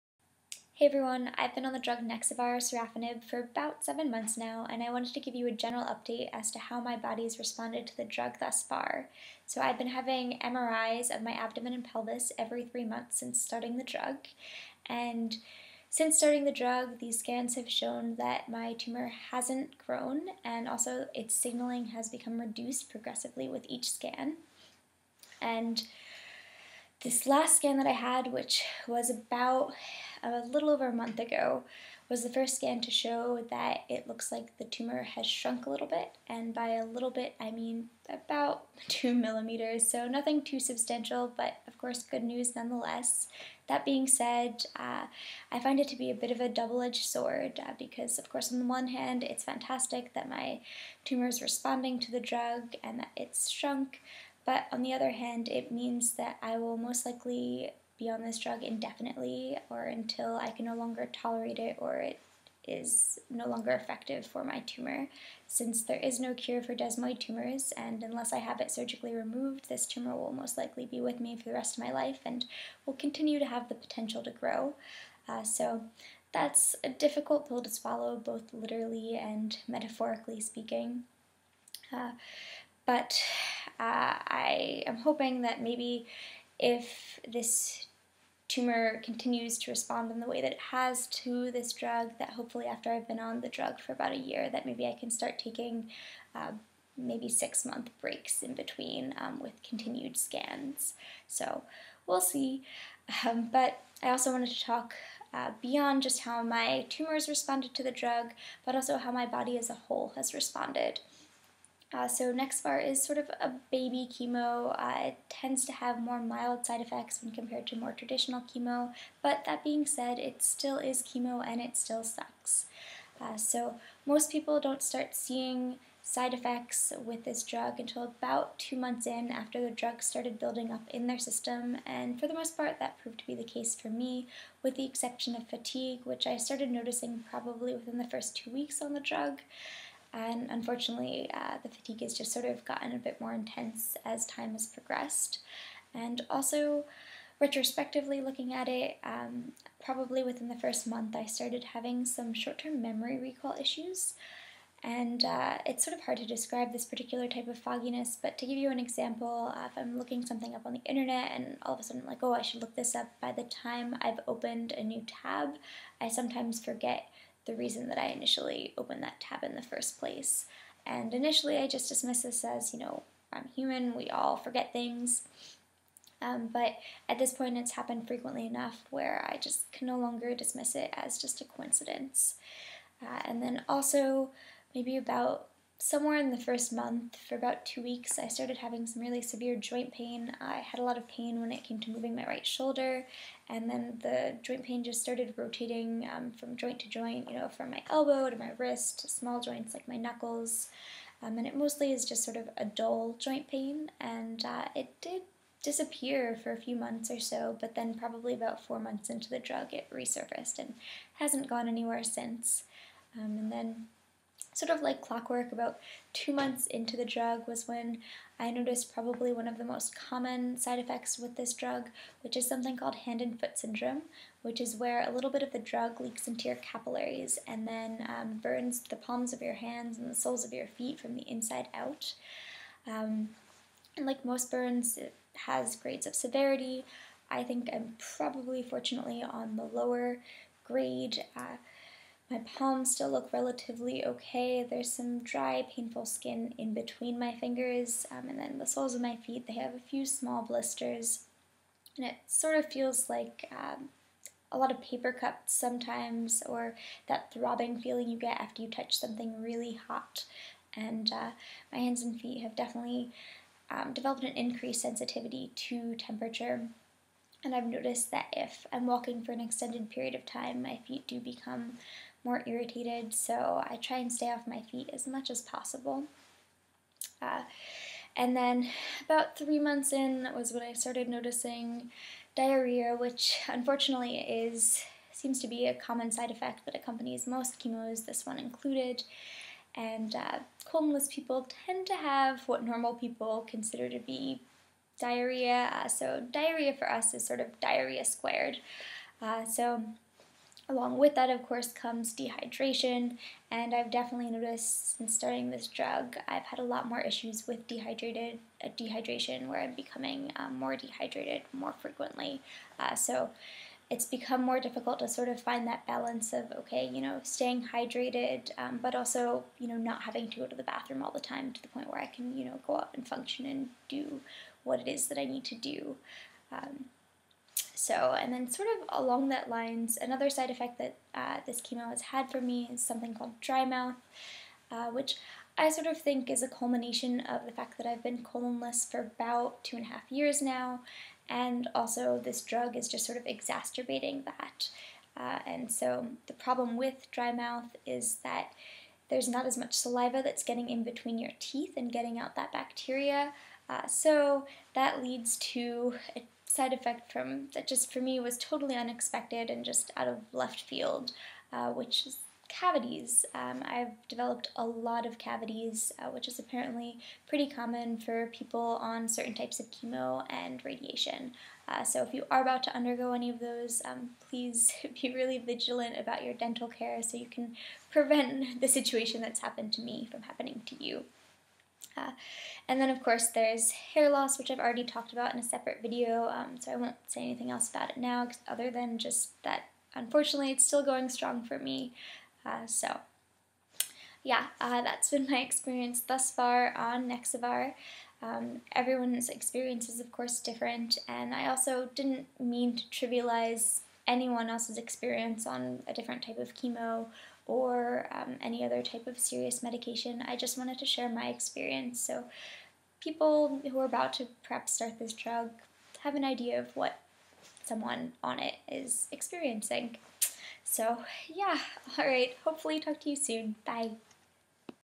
Nexavar Narrative: 7 Month Update
I was asked to give a general update video about my body's response to the drug Nexavar thus far so here it is, side effects and all!

Not enough detail? Too much? Tired of hearing about desmoids (Me too!)? Let me know what videos you'd like to see by commenting below or messaging this channel!